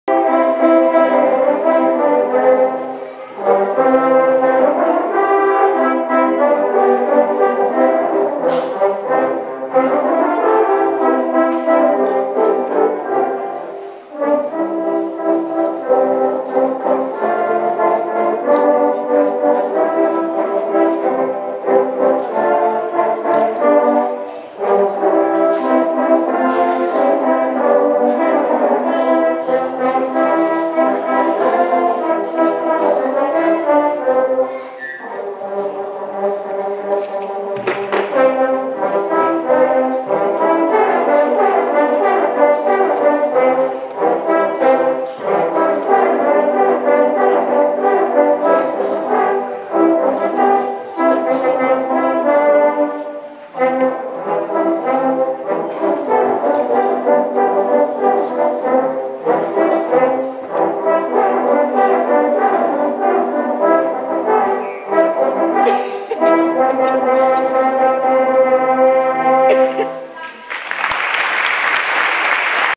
20131201 Cloenda 6a Trobada Trompistes Catalunya (1/2)
Peça interpretada al final del concert de cloenda de la 6a Trobada Internacional de Trompistes a Catalunya pels estudiants més joves del Grau Professional